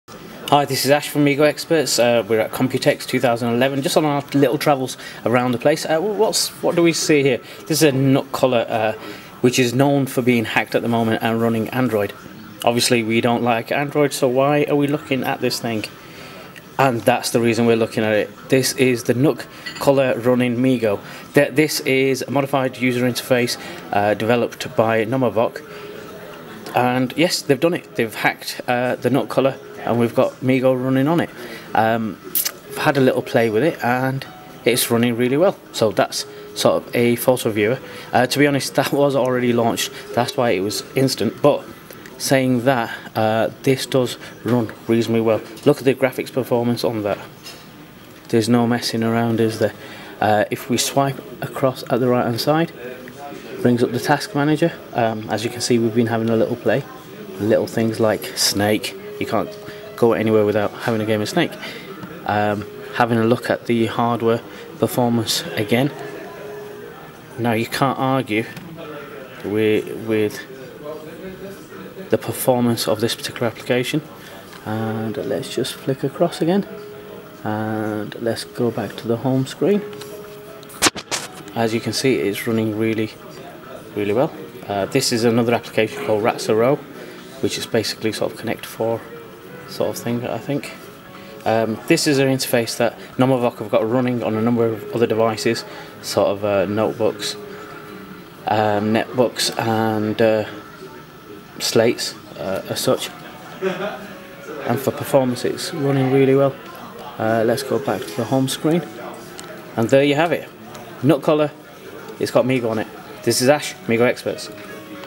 Nook Color gets Hacked again - Now its MeeGo
Nook Color is fast becoming the most Hackable tablet device out there. First Android and now MeeGo. Good stuff !